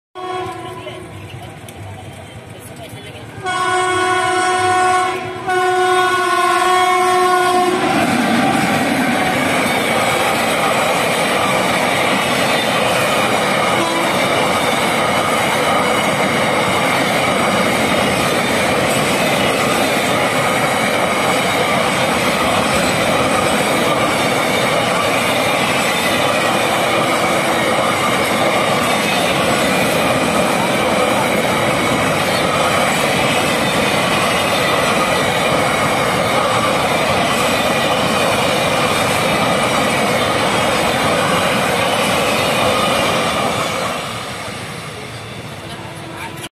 CONCOR Container Wagons with Valsad (BL) WAG 7 going towards Indore Junction || BLC Wagons || IR
CONCOR Container Wagons coming from Tihi Railway Station Container Depot and going towards Indore Junnction
Locomotive - 28637 Valsad (BL) WAG 7
Location - Reti Mandi Railway Crossing, Rajendra Nagar, Indore
Date - 10/12/2020